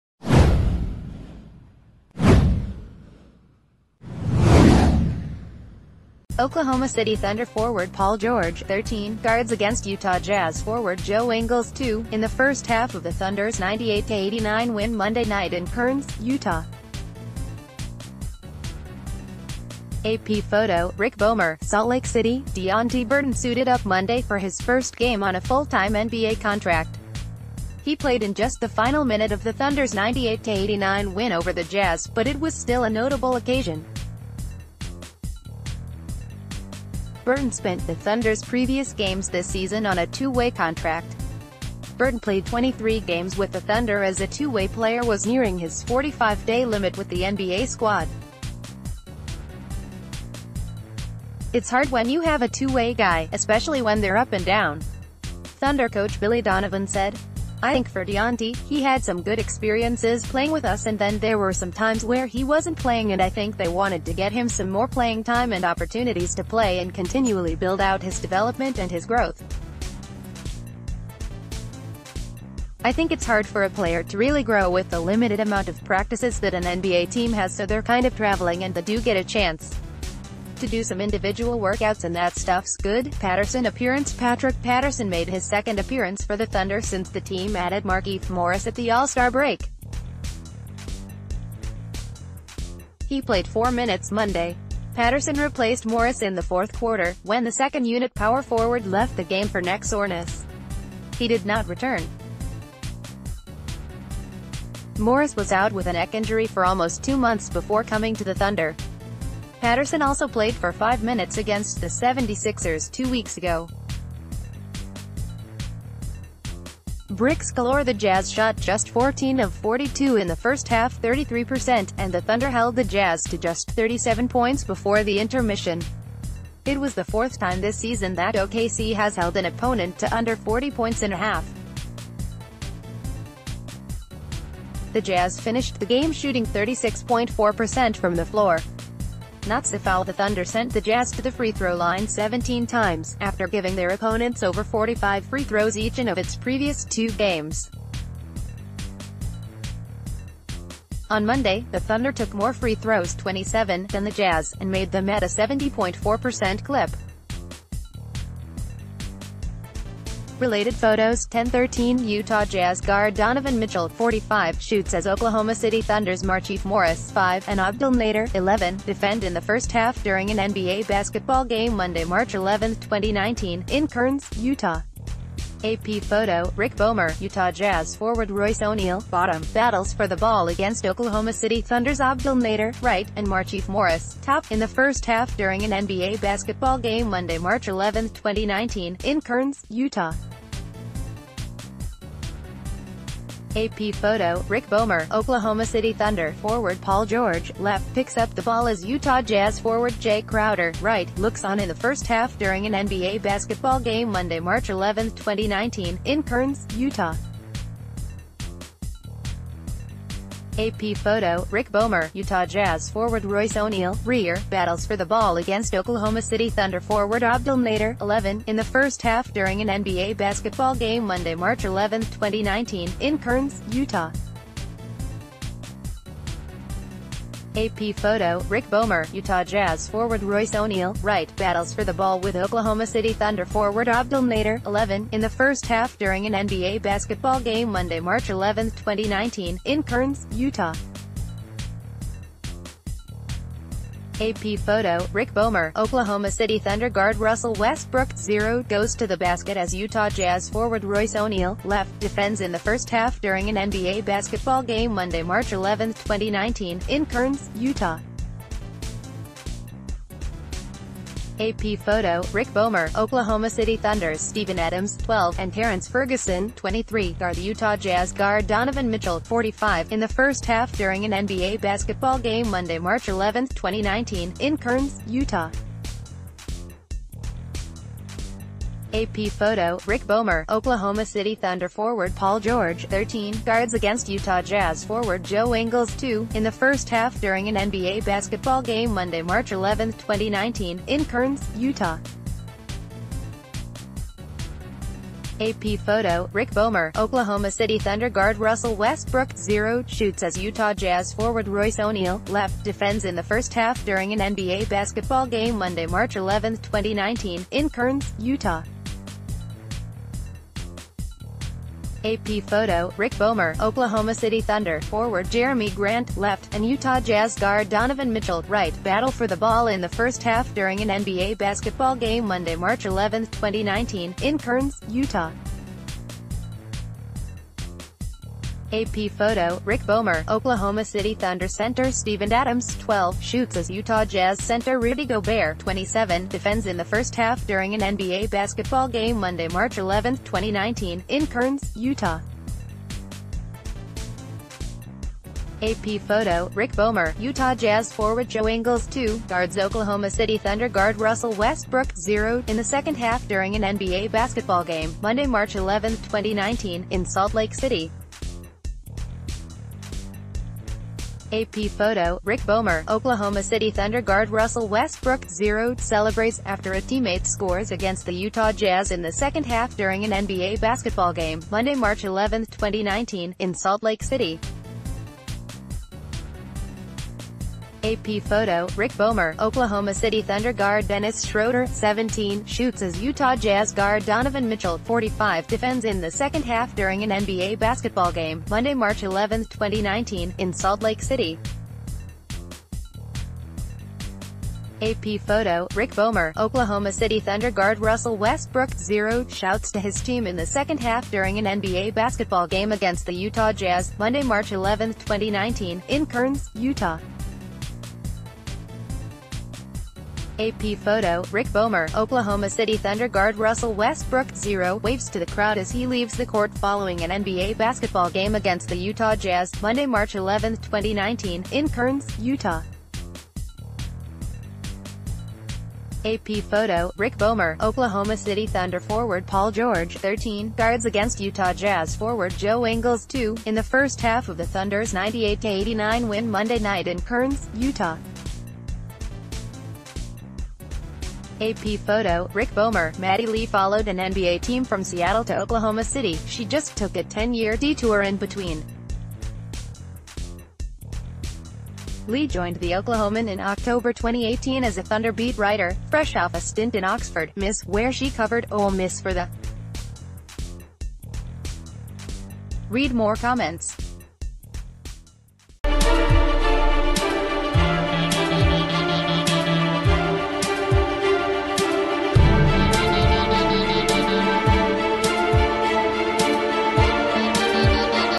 Thunder journal: Deonte Burton plays his first game on an NBA contract
Oklahoma City Thunder forward Paul George (13) guards against Utah Jazz forward Joe Ingles (2) in the first half of the Thunder's 98-89 win Monday night in Kearns, Utah. [AP Photo/Rick Bowmer] SALT LAKE CITY -- Deonte Burton suited up Monday for his first game on a fulltime NBA contract. He played in just the final minute of the Thunder’s 98-89 win over the Jazz, but it was still a notable occasion. Burton spent the Thunder’s previous games this season on a two-way contract. Burton played 23 gam...